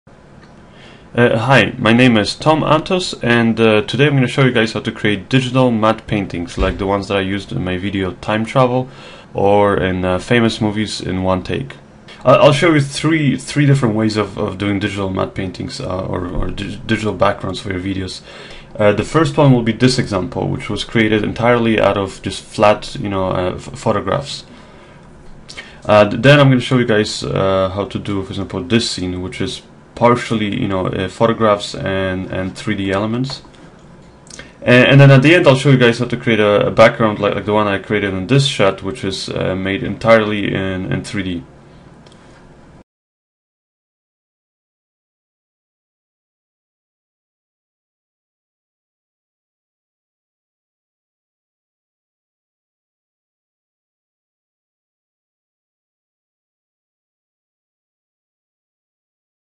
Digital Matte Painting Tutorial
Learn how to create matte paintings digitally that you can use in your next video. Programs I use are Adobe Premiere for editing, Adobe After Effects for compositing and fx's, as well as Maya 2012 for all the 3D elements.

or